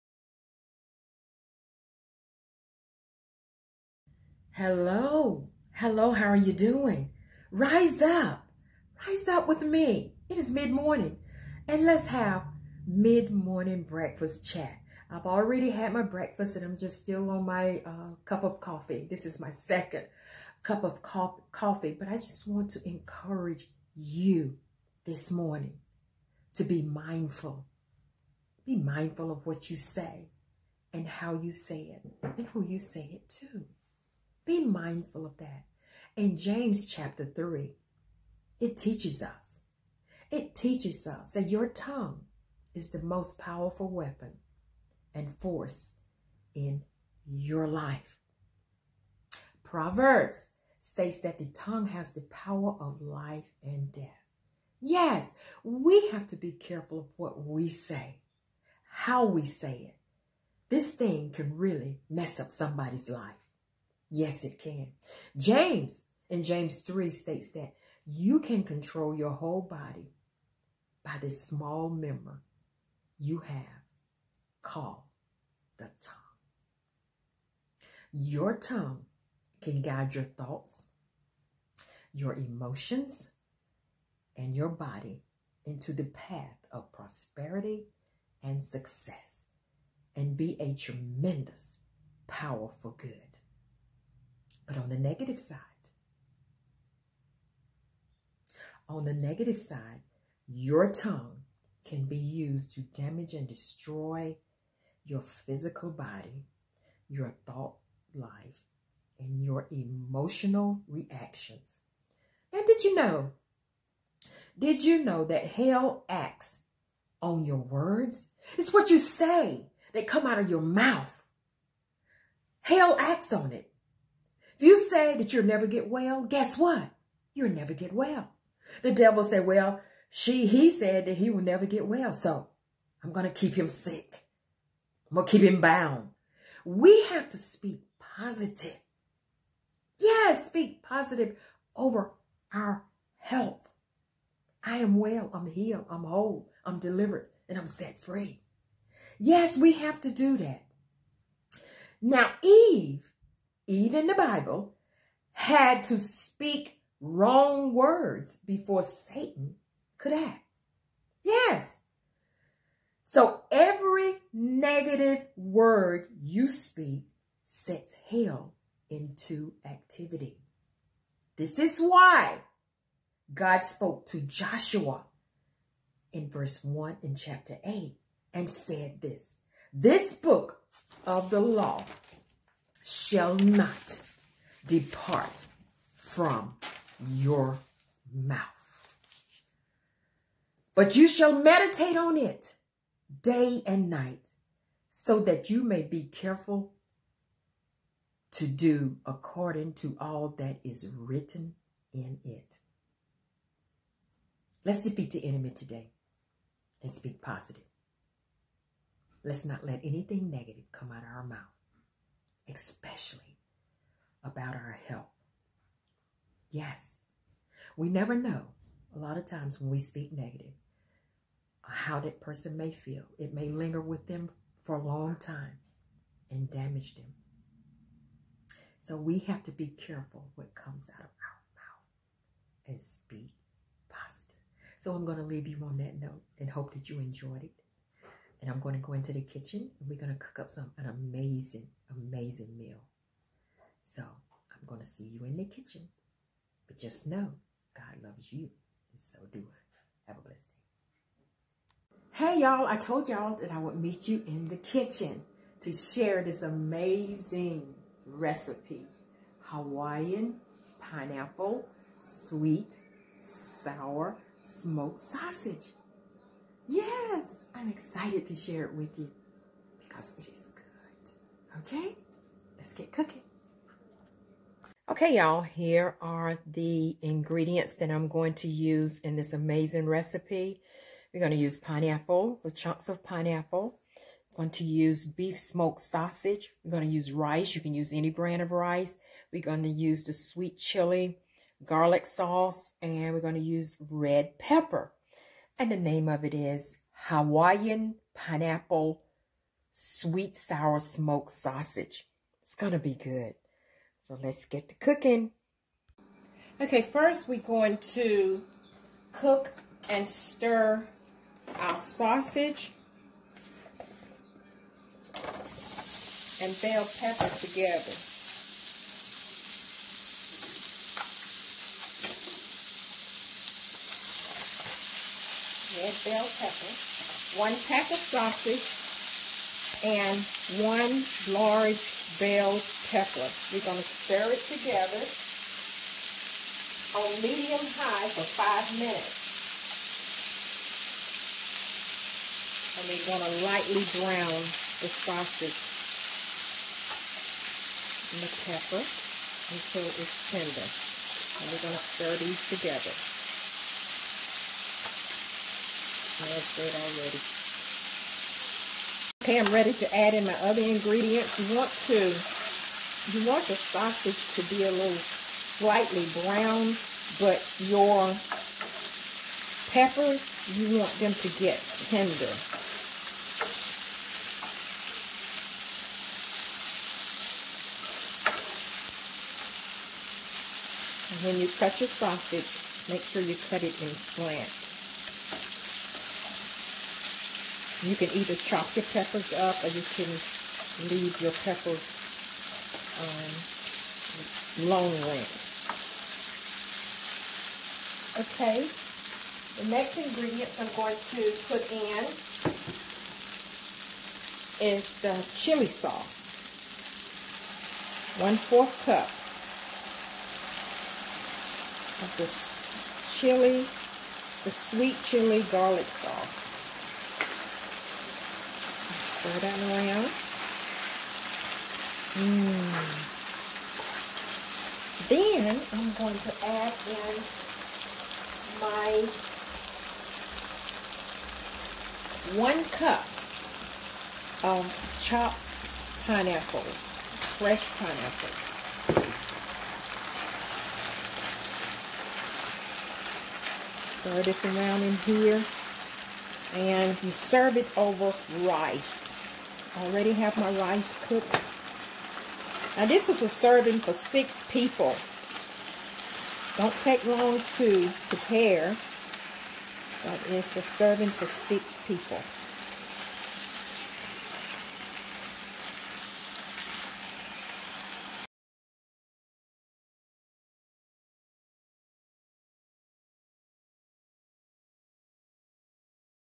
"The Tongue" + Hawaiian Pineapple Sweet Sour Smoked Sausage
1 pkg Smoked Sausage, 1 red bell pepper, chopped, 1/4 cup sweet chili garlic sauce, 1 cup fresh pineapple, cut into 1/2 inch pieces, 3 cups cooked rice

Cook and stir sausage and bell peppers over med.-high heat for 5 min. until meat slightly brown and peppers are tender

Add chili garlic sauce and pineapple; cook 5 min and serve over rice.